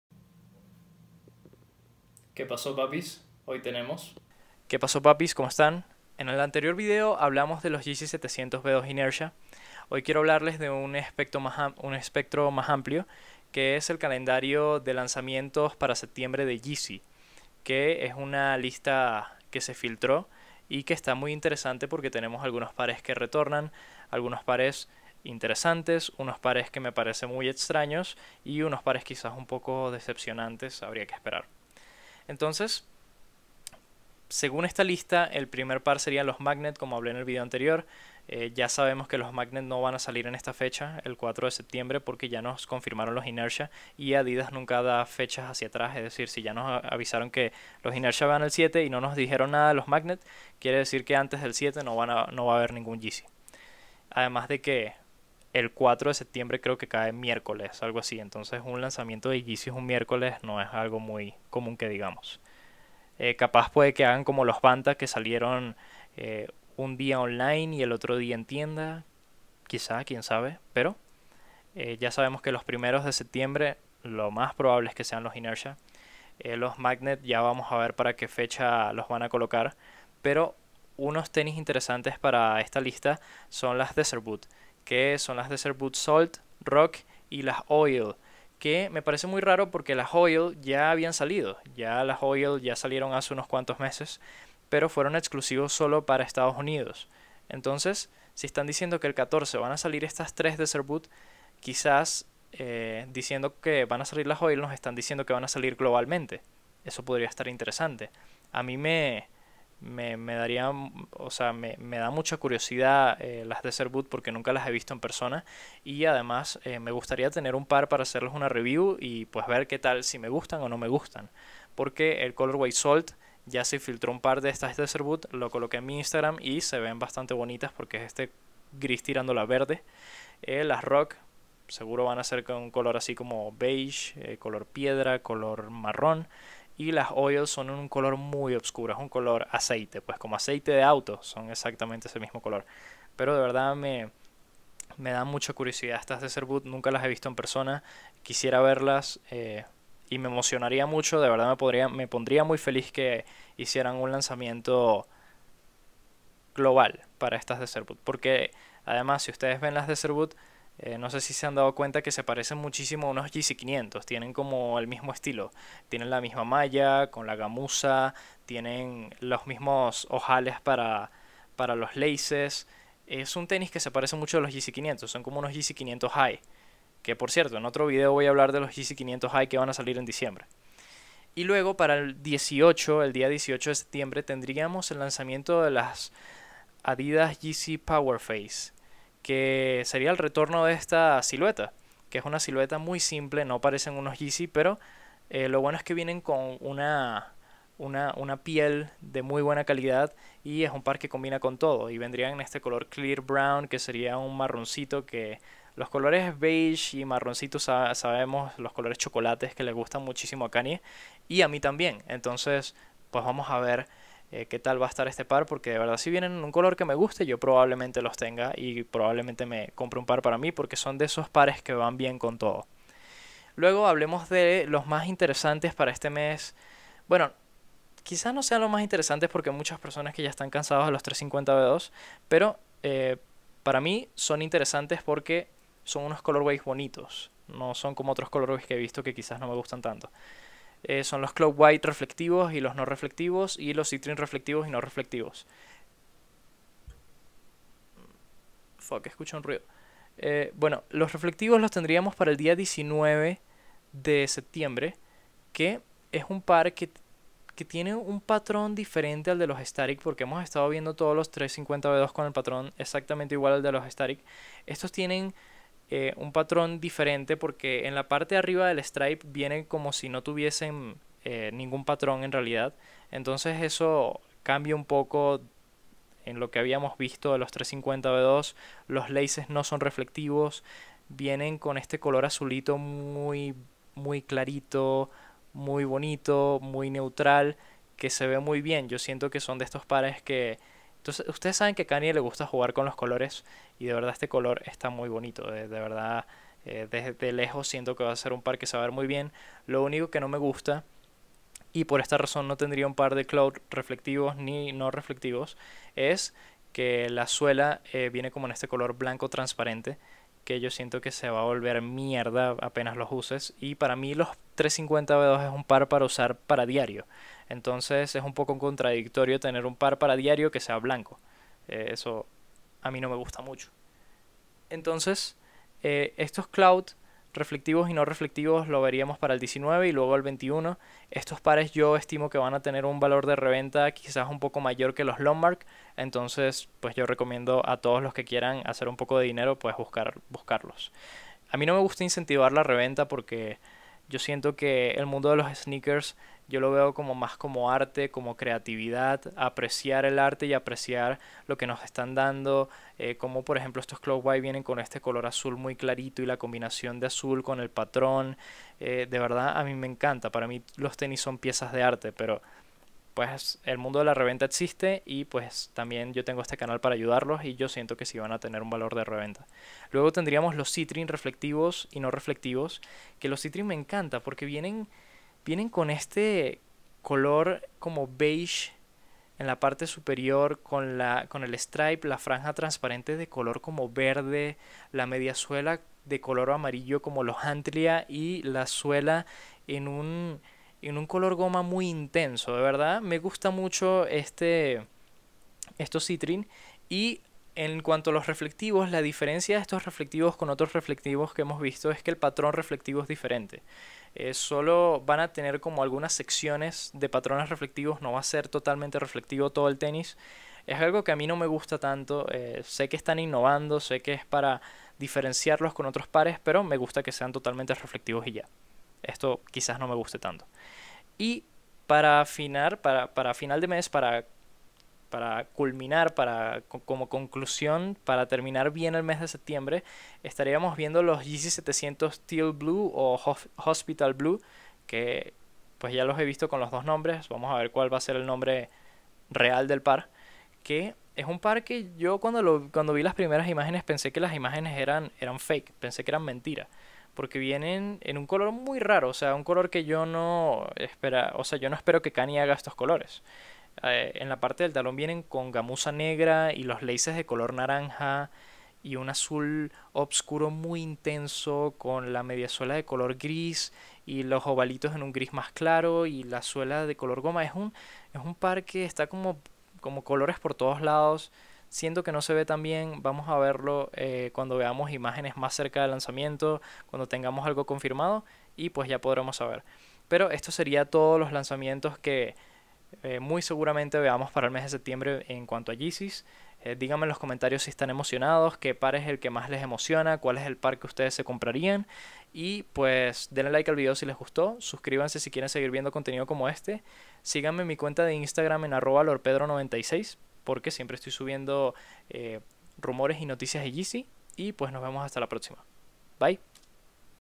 LANZAMIENTO DE YEEZY PARA SEPTIEMBRE - ¡Más de 11 pares!
Tendremos un mes muy emocionante, algunos pares interesantes, otros no tanto lol

Esperemos las Desert Boots tengan un lanzamiento Global, porque me gustan muchísimo